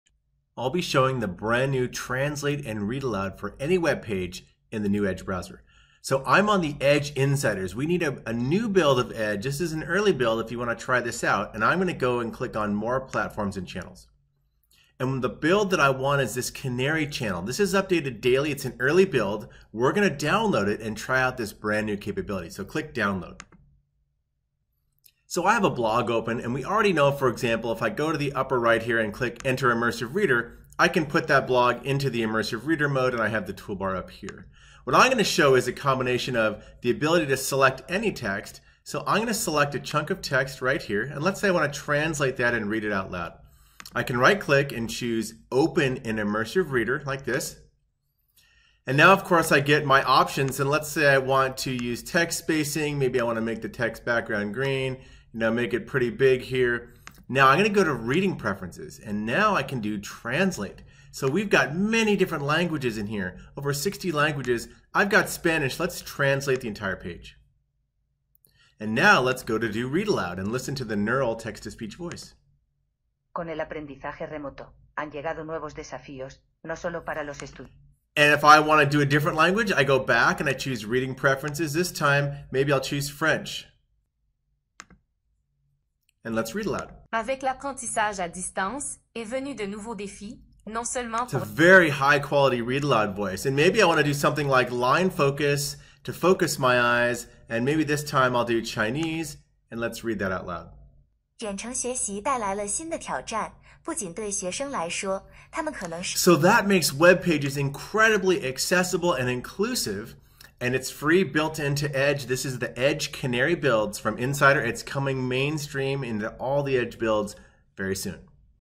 How to translate and Read Aloud a web page with Immersive Reader in the Microsoft Edge browser
A step-by-step tutorial on how to translate a web page (or a part of web page) and then read aloud with the Immersive Reader in the Microsoft Edge browser. Edge is the most inclusive and accessible browser in the world!